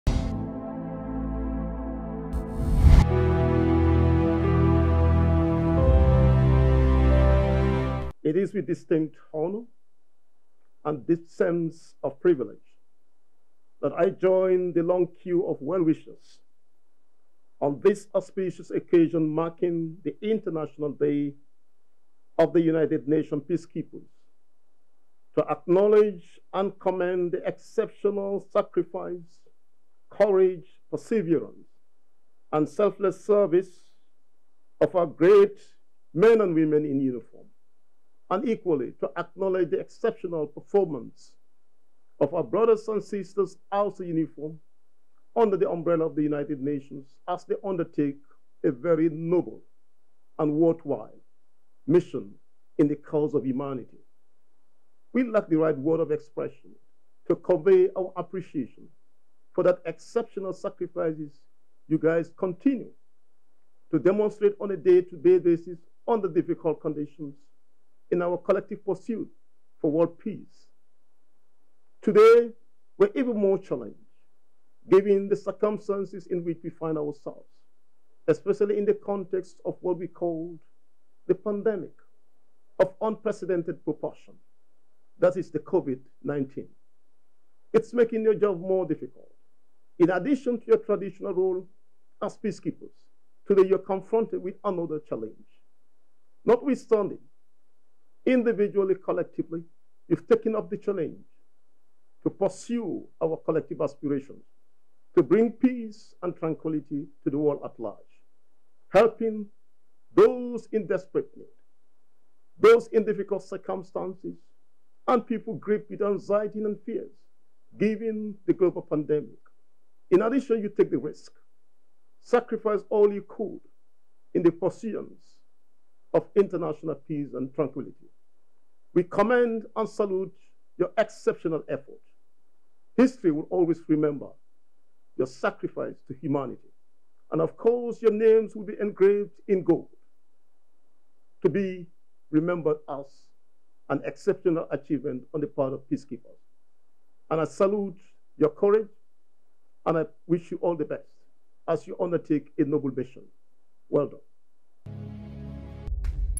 EnglishNews
29 May, offers a chance to pay tribute and honour more than 3,900 peacekeepers who have lost their lives serving under the UN flag since 1948, including 102 last year.
This year, the challenges and threats faced by peacekeepers are even greater than ever, having not only to cope with the COVID-19 pandemic, but also support and protect the people in the countries they are based in.
With this year’s Theme: Women in Peacekeeping: A Key to Peace The Chief of Defence Staff of the Gambian national army,  in a special message to mark the day said they are more challenge today in maintaining peace around the world.